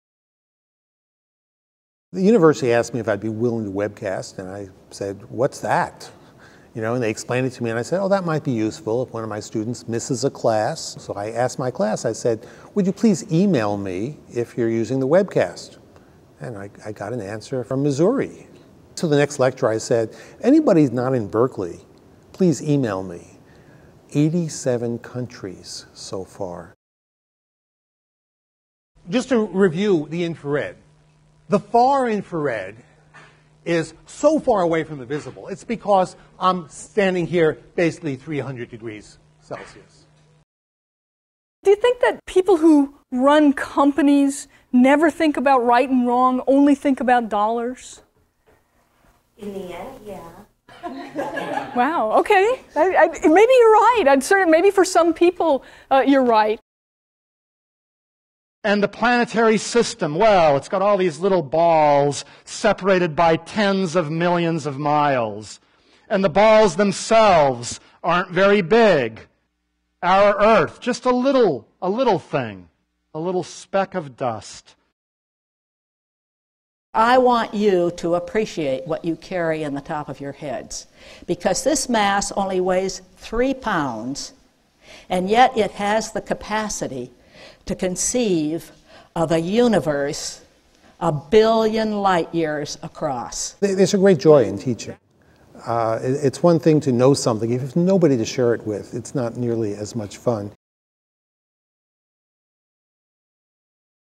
What's webcast.berkeley?
A quick overview of webcast.berkeley with insights from Professor Richard A. Muller.